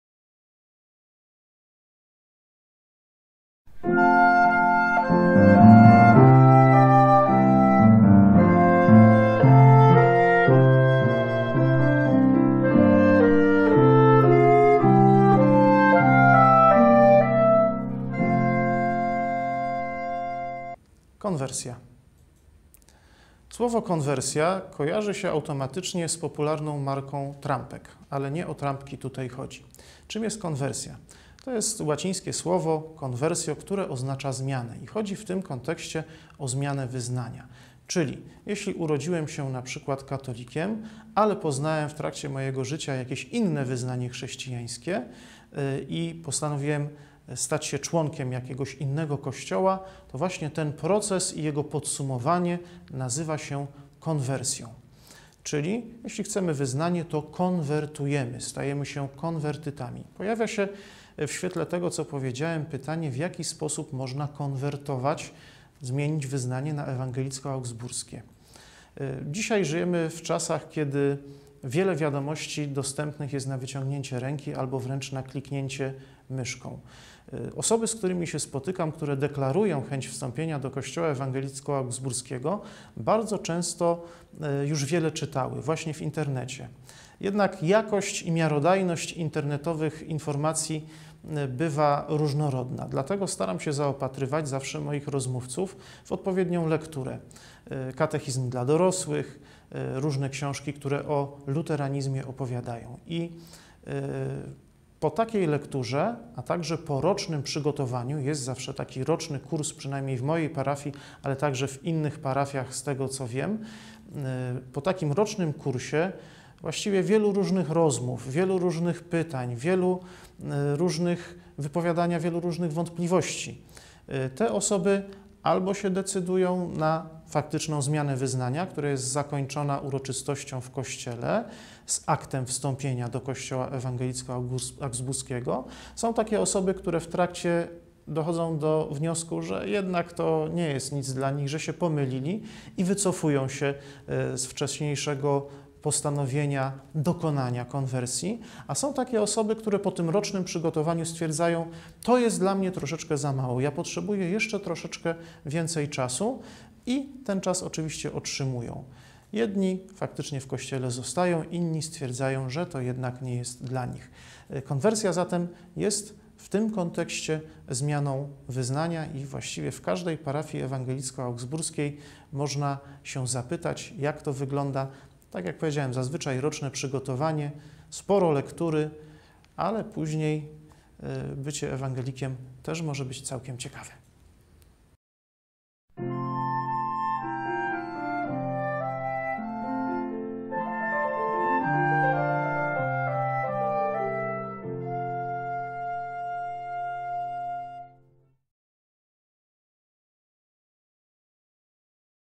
Ewangelickie ABC: Konwersja
Kanał @LuteraniewWarszawie zapraszają do obejrzenia kolejnego odcinka z serii Ewangelickie ABC. Tym razem na pytanie czym jest konwersja - odpowiada ks. Wojciech Pracki z Parafii Ewangelicko-Augsburskiej w Opolu.